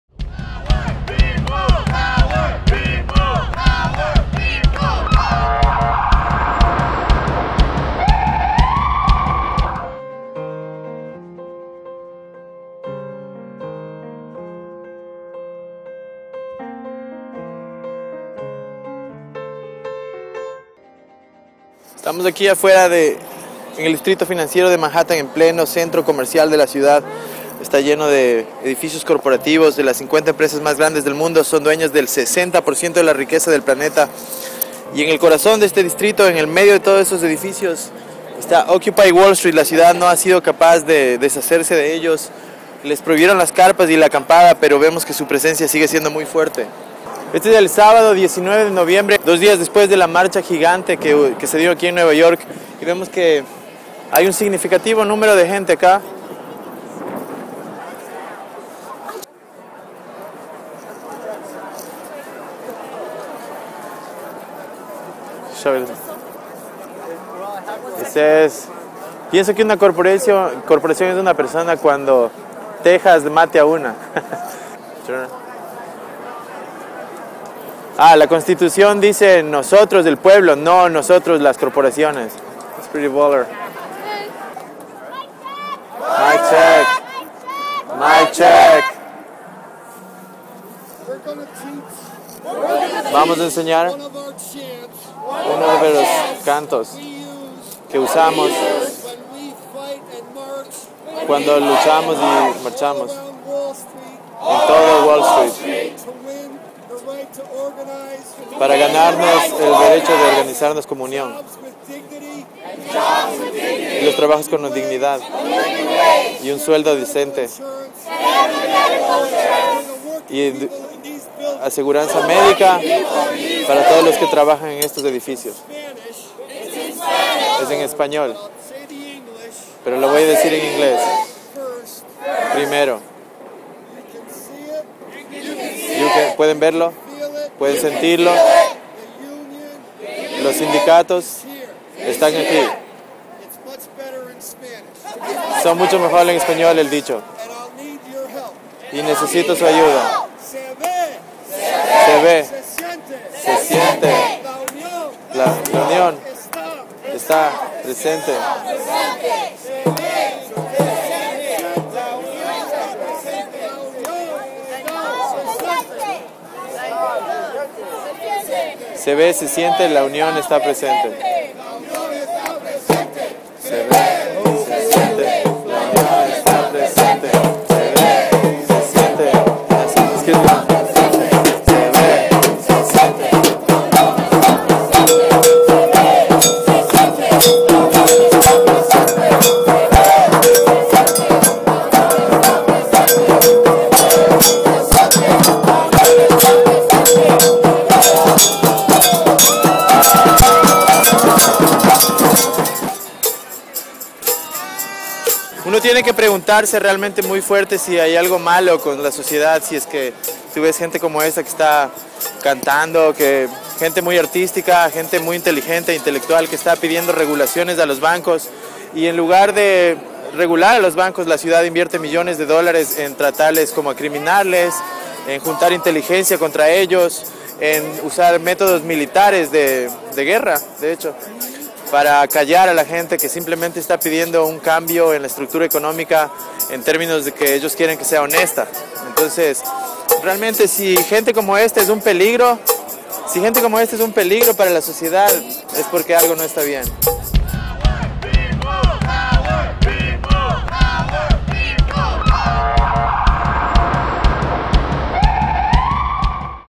Occupy USA / Manhattan Sabado 19 de Noviembre 2011
Reportaje sobre el ambiente en Liberty Plaza la capital del movimiento Occupy cuya acampada fue arrasada pocos dias antes / Occupy USA - El Otoño Americano